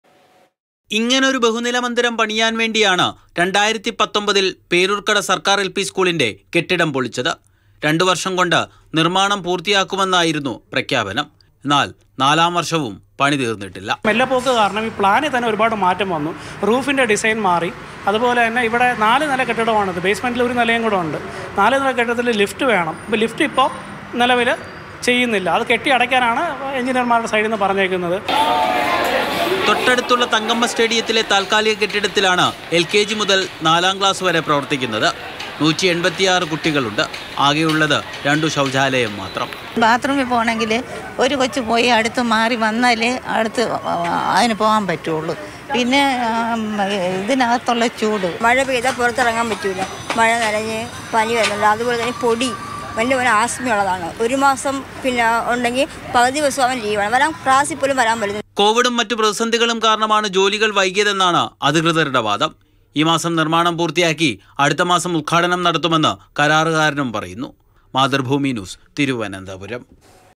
നാല് വർഷമായി, പേരൂർക്കട സർക്കാർ സ്കൂൾ കെട്ടിട നിർമാണം ഇപ്പോഴും പാതിവഴിയിൽ | Government School
നാല് വർഷമായി, പേരൂർക്കട സർക്കാർ സ്കൂൾ കെട്ടിട നിർമാണം ഇപ്പോഴും പാതിവഴിയിൽ

Happy viewing!